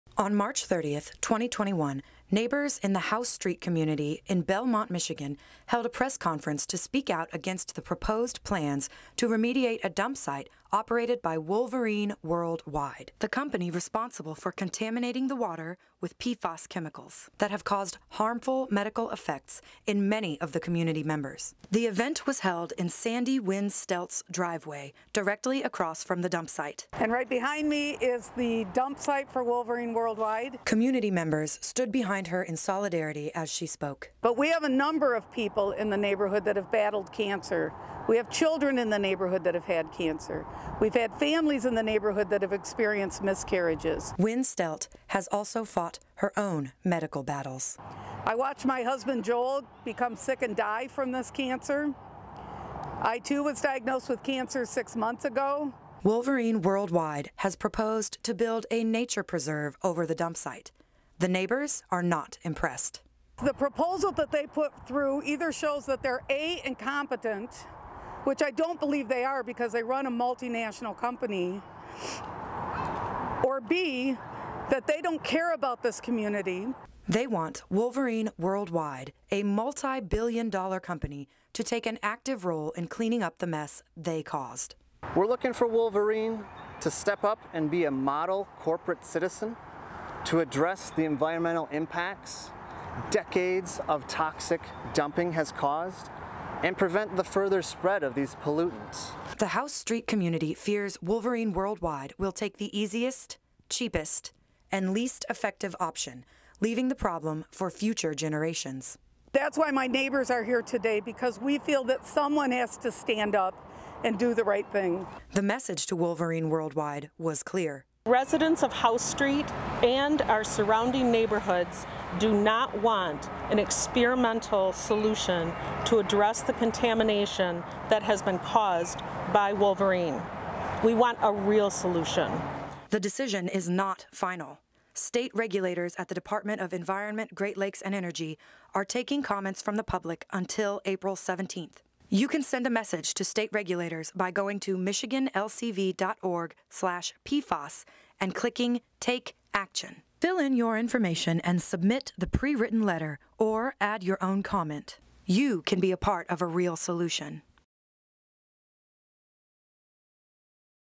House Street Press Conference on Wolverine World Wide and Proposed PFAS Remediation Plan
Neighbors of House Street held a press conference on March 30, 2021 to express their disapproval for the remediation plan proposed by Wolverine World Wide, the company responsible for contaminating the area with PFAS chemicals.

Wolverine's proposal to build a nature preserve on the contaminated site is but a band-aid on a bullet hole. The community, victim to various forms of loss and illnesses, demand a real solution.

Michigan's Department of Environment, Great Lakes, and Energy is accepting public comments until April 17.

to take action and send state regulators a message.

Be a part of a real solution.